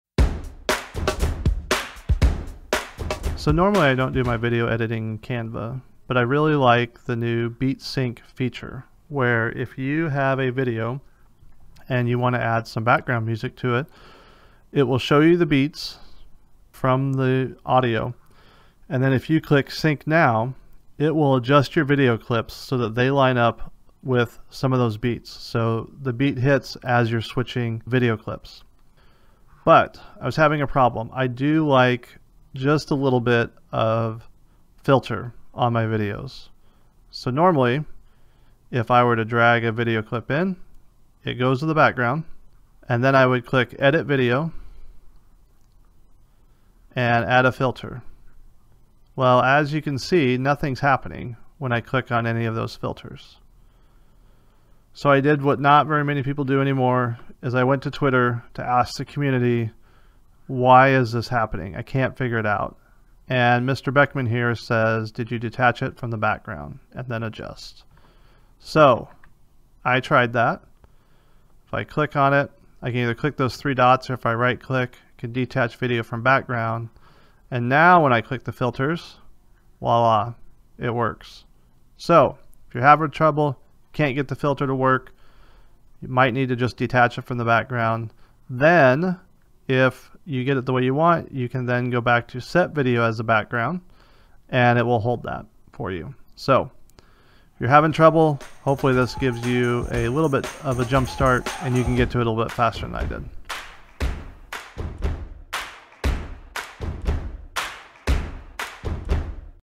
Filters in Canva Videos
Are you editing video in Canva and can't get the filter to work? I may have a solution!
0:00 Intro
0:03 Beat Sync
0:31 Not being able to add filters